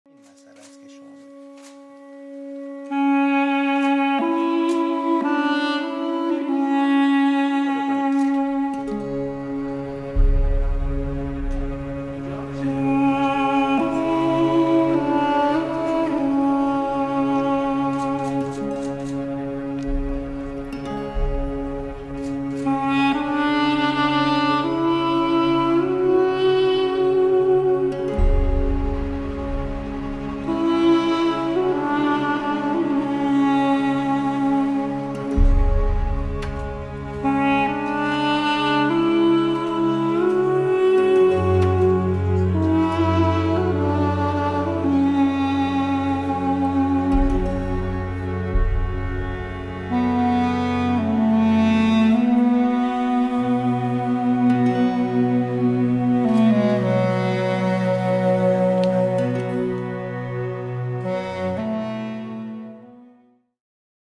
Thousands of rare books preserved, digitised at Astan Quds Ravazi library in Iran
The Astan Quds Manuscript Library, a landmark collection containing tens of thousands of rare books, is keeping up with the times thanks to a digital archive project.